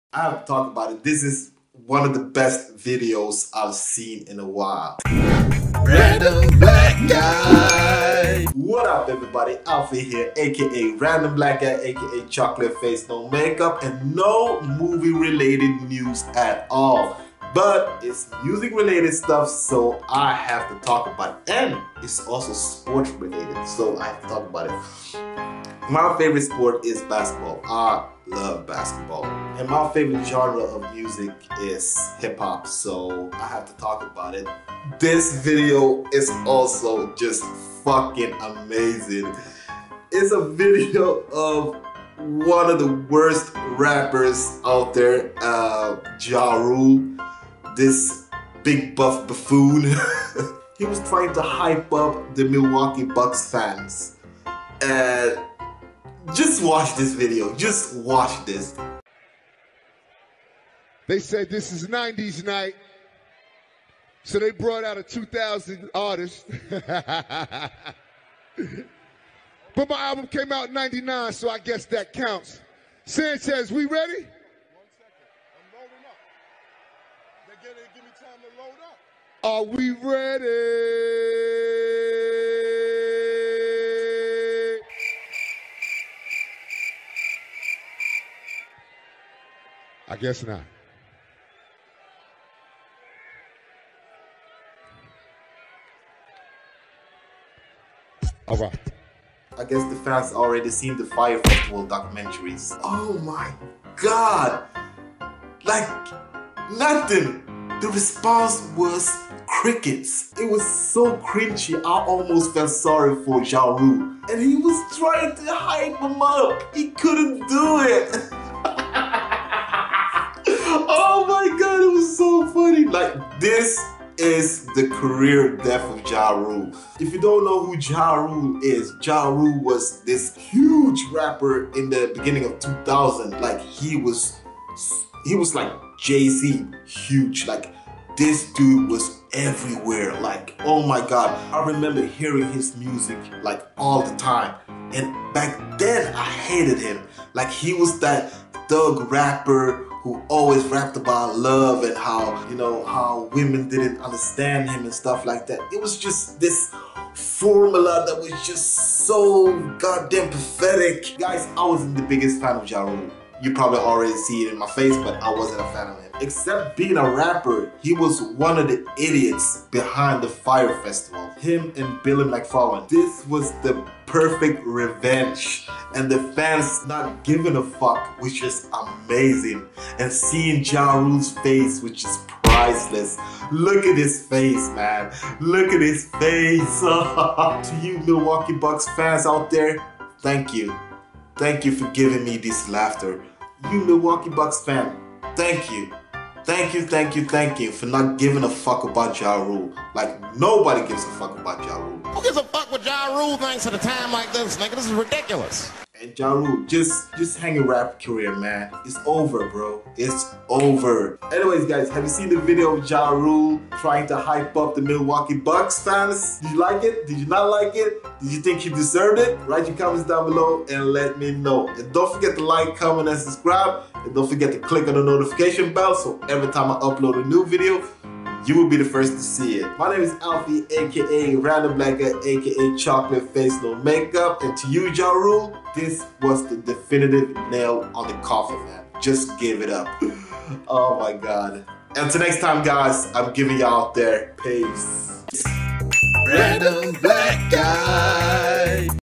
DEAR JA RULE...👀👀
Ja Rule tries to hype up a crowd at Milwaukee Bucks/Minnesota Timberwolves game, fails as bad as Fyre Festival.